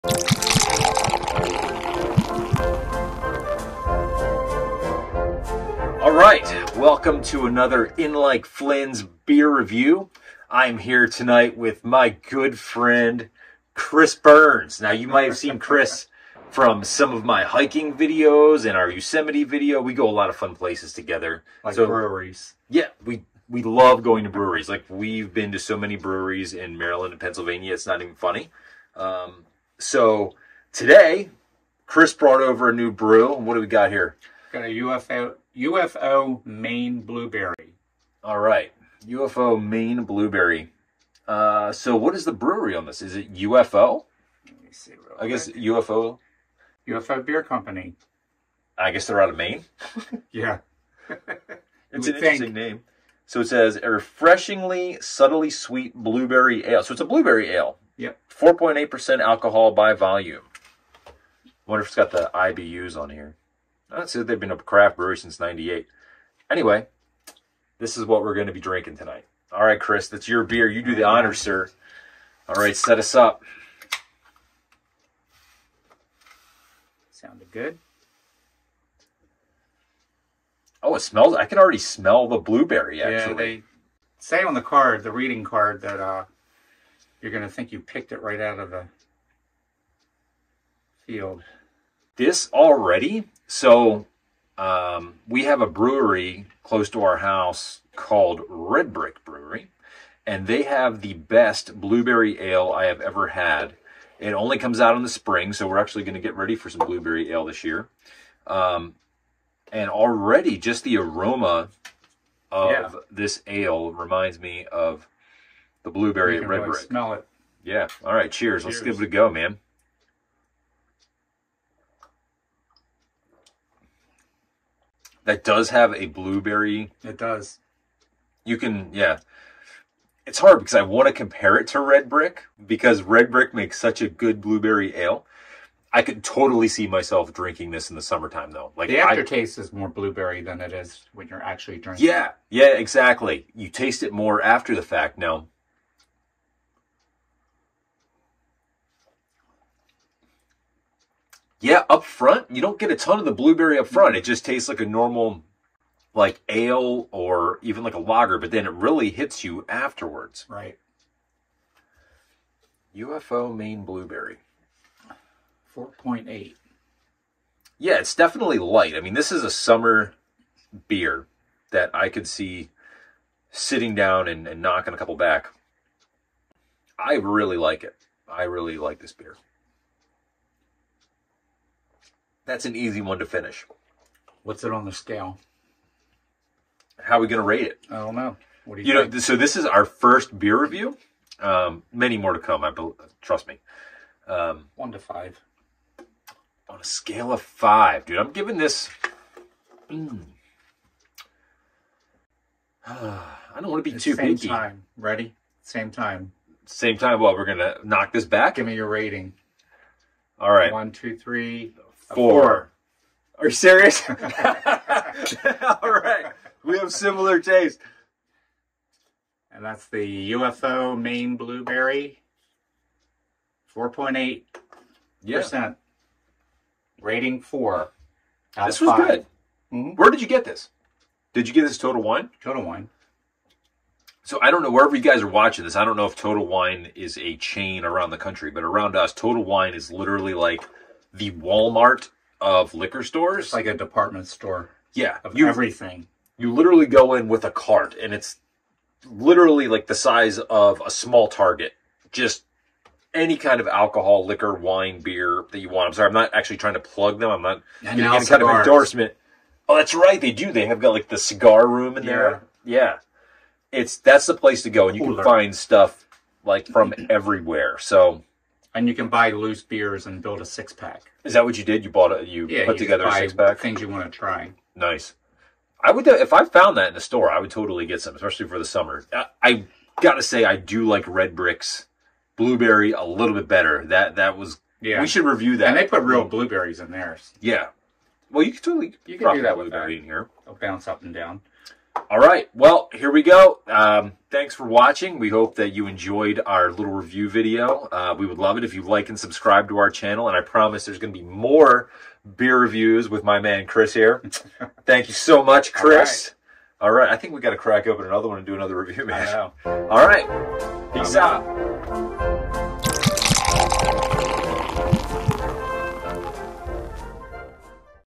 UFO Beer Company Maine Blueberry Ale | Full Tasting and Review | 4K
Who doesn't love a good refreshing Blueberry Ale in the summer time? We know we sure do.  But how does this one stack up to our normal Blueberry go-to's? At 4.8% ABV you know this will go down nice and smooth.  We will review this tasty beverage in the video.  Join us for a copy good time at the bar!